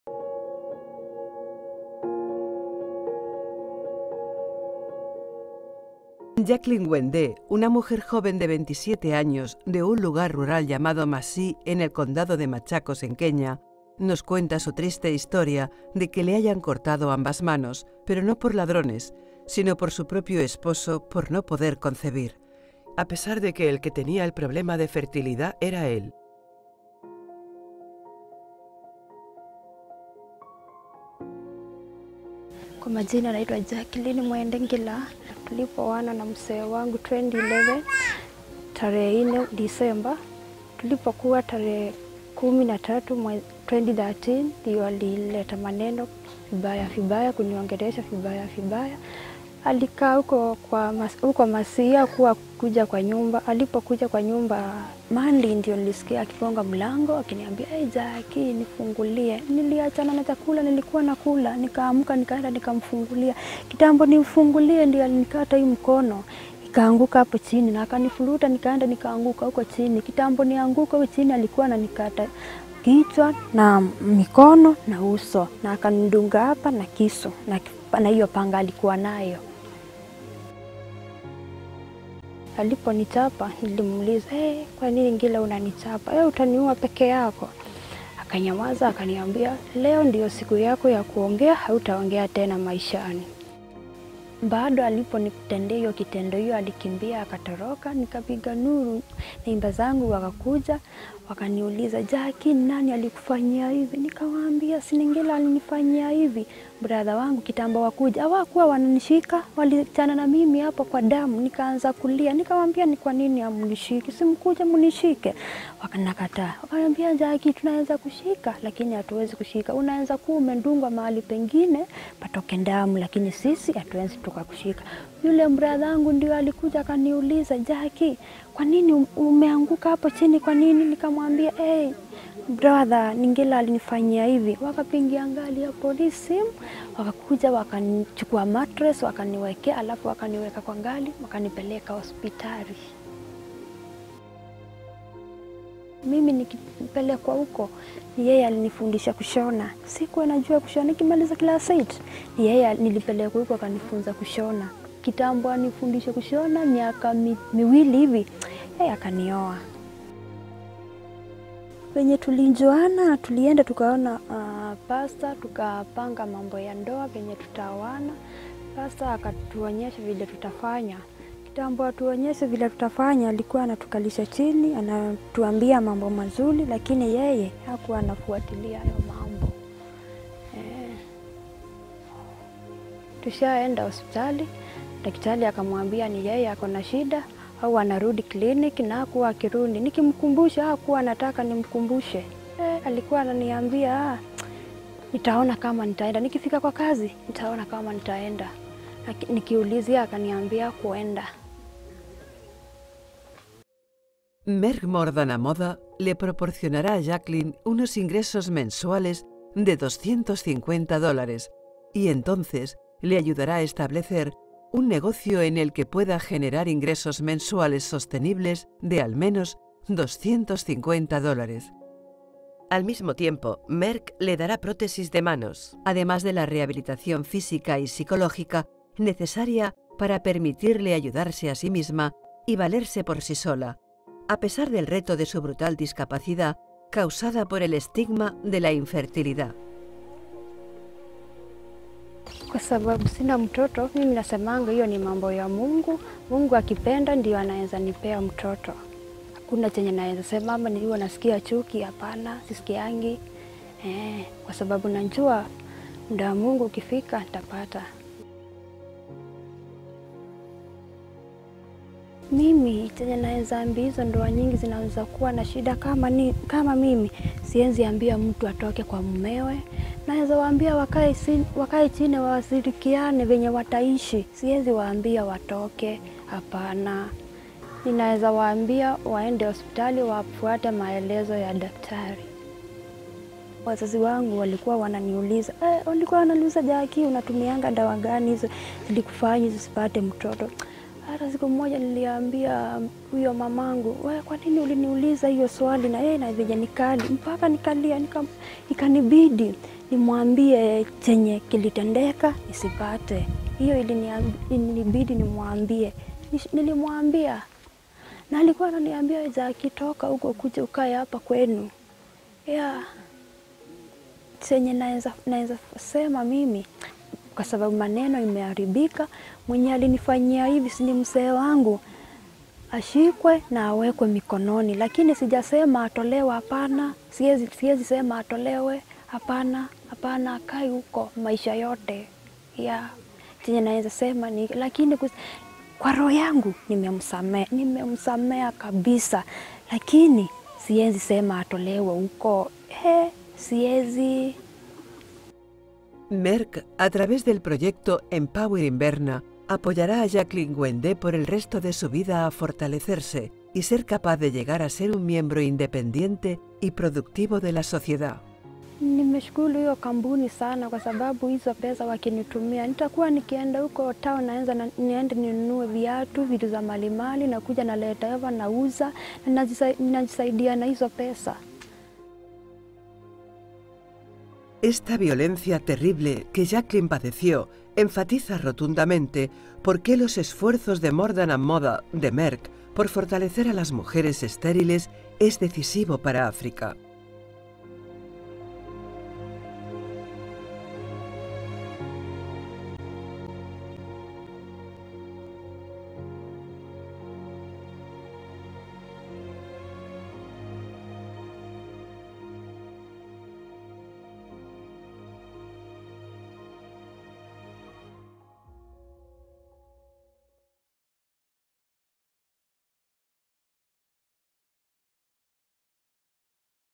*Merck More Than A Mother Heroine, Jackline Mwende from Kenya shares her story in Spanish
This video is not intended for US / Canada audience.

"I would like to invite all of you to watch the story of Merck More Than A Mother Heroine, Jackline Mwende from Kenya.

Jackline Mwende, a young woman of 27 years from Masii, Machakos County in Kenya had her hands hacked off - not by thieves but by her own husband for failing to conceive, even though he was the one with fertility problems. Merck Foundation aims at empowering infertile women economically and socially across Africa.

As the CEO of Merck Foundation and as an African women, I am proud that Merck More than a Mother initiative continues to support Jackline Mwende, a victim of infertility stigma, throughout the rest of her life after having her hands brutally chopped off for failing to bear children after seven years of marriage. Merck Foundation also aims to empower infertile women across the continent through access to information, health and change of mind-set through the 'Merck More Than a Mother' initiative."

~Dr. Rasha Kelej,
CEO of Merck Foundation & President of Merck More Than A Mother
One of 100 most Influential Africans (2019)